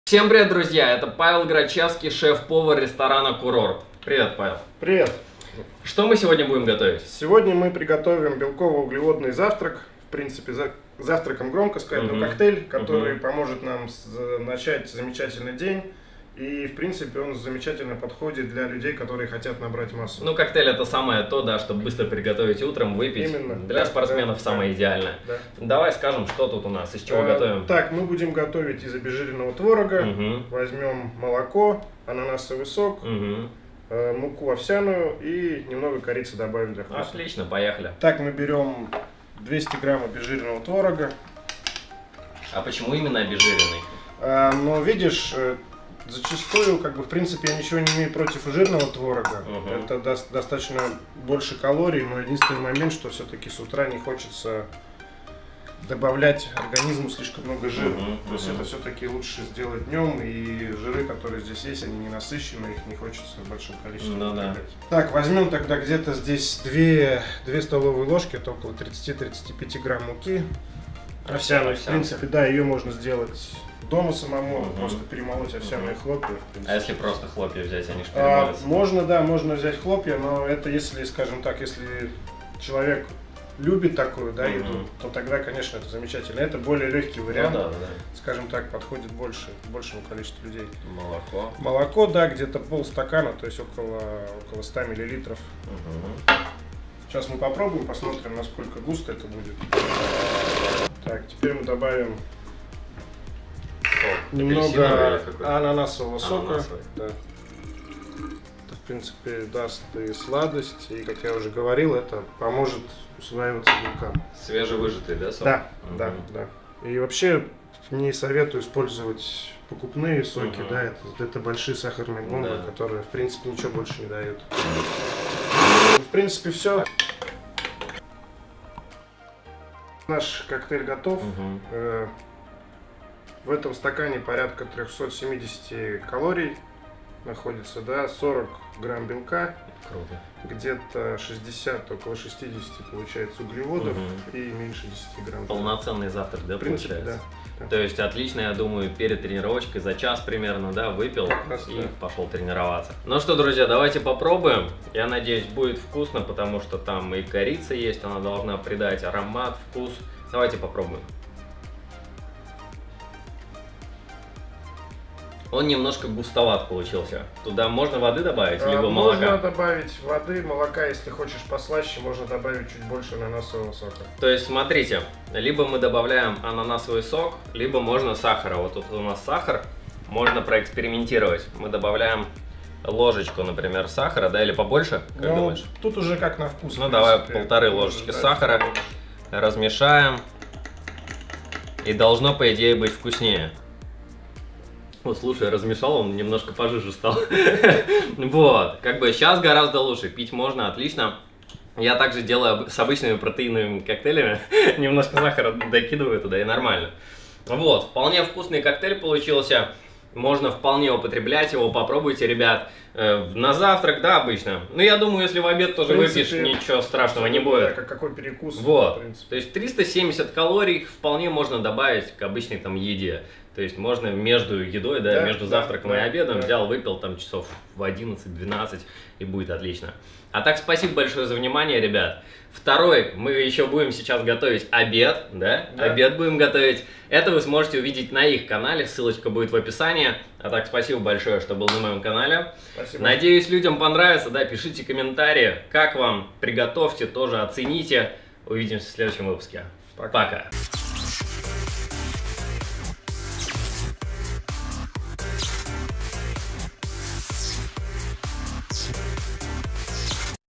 Углеводный коктейль для дрищей! Легкое приготовление. Лучше протеина.
В этом видео вы увидете, как БЫСТРО приготовить вкусный, высоко-углеводный коктейль, который можно пить как утром, так и между приемами пищи! Готовим с шеф-поваром!